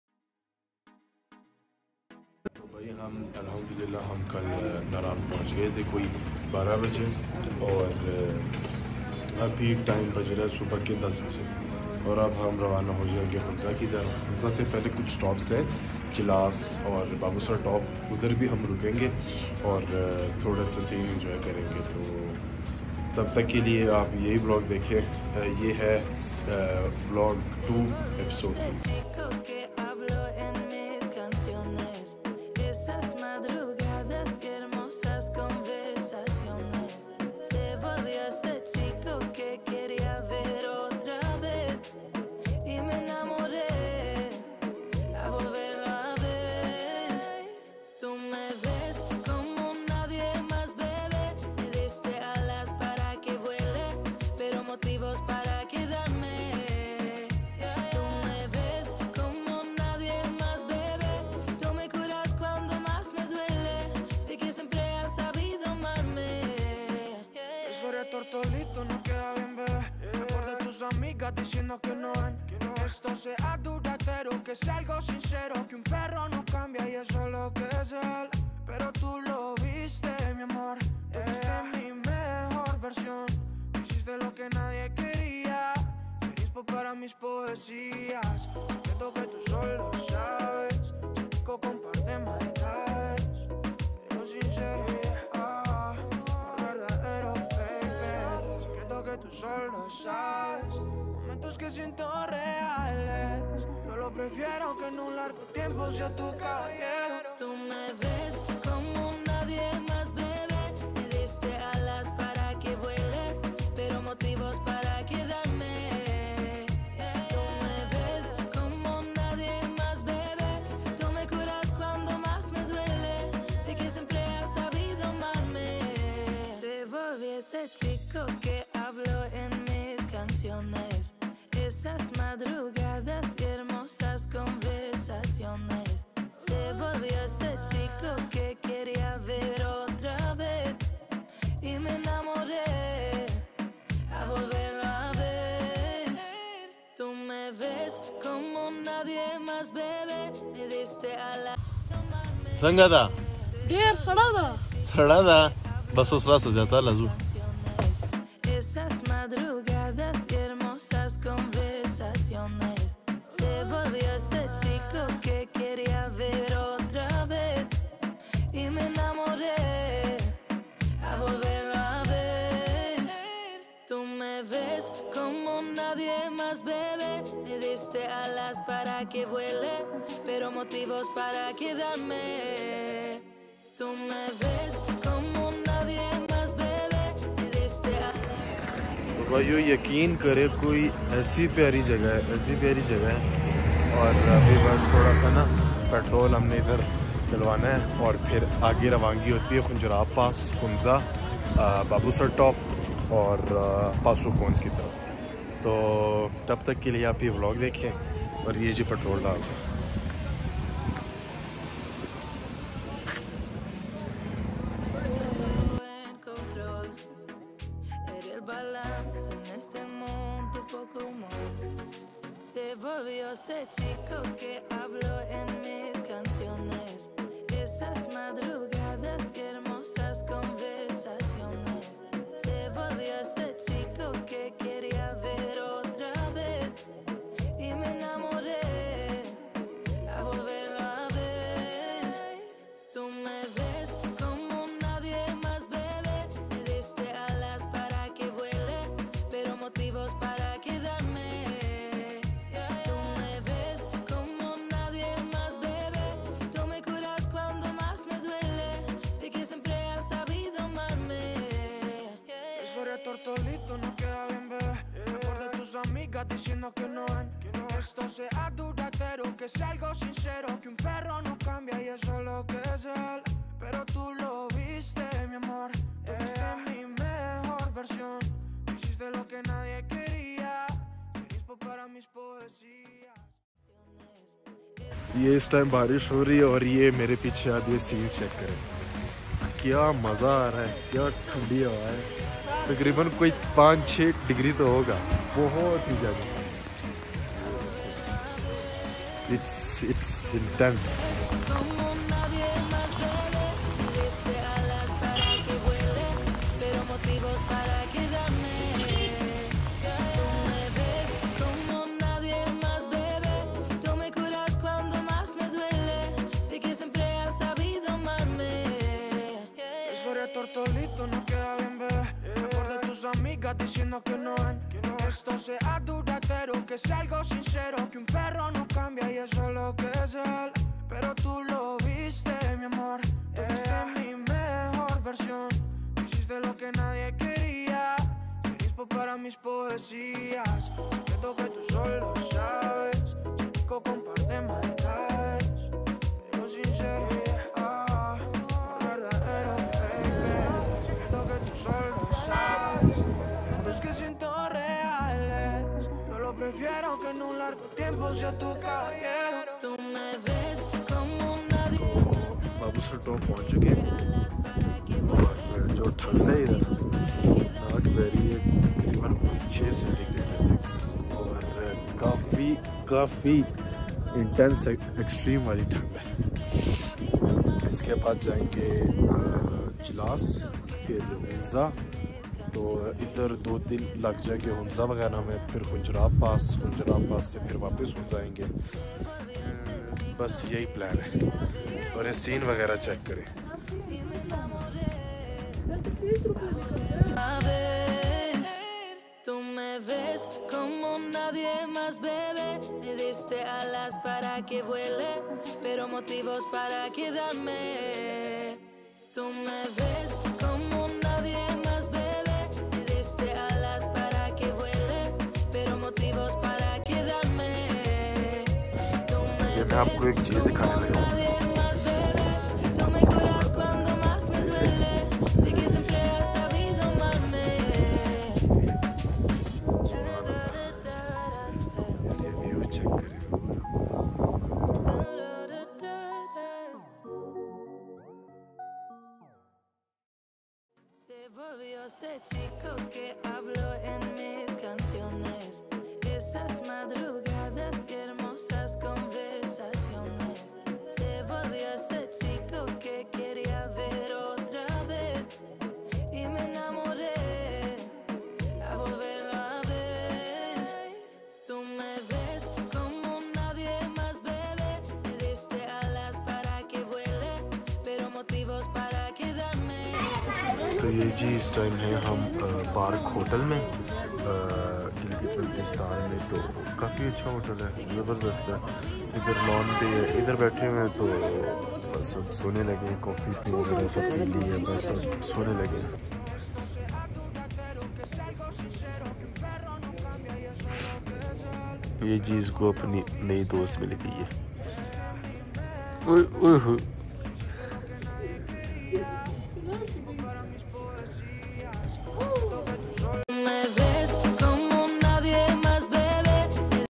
Ep: 2 | Northern areas of Pakistan | Gilgit | Naran to Gilgit
In this beautiful video we started our journey from faisalabad to all the way Khunjerab pass and in this video it’s from naran to Gilgit baltistan . Waiting for your enormous support ❤️